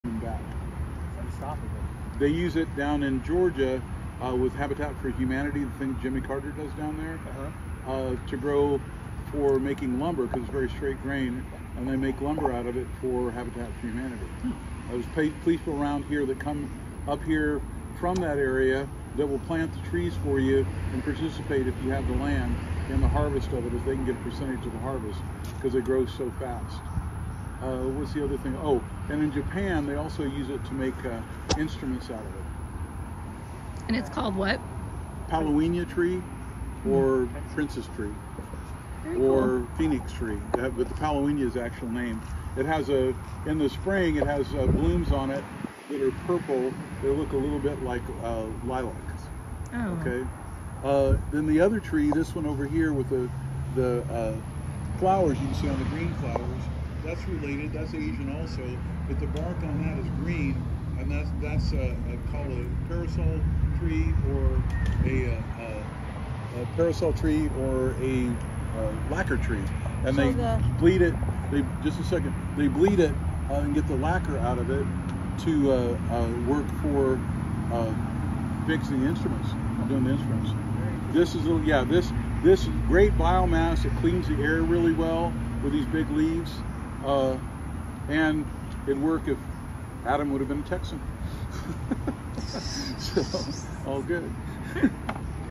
Conversations with James: The Paulownia Tree
Chatting about the benefits of Paulownia Tree. It's also called the Princess Tree or the Empress Tree.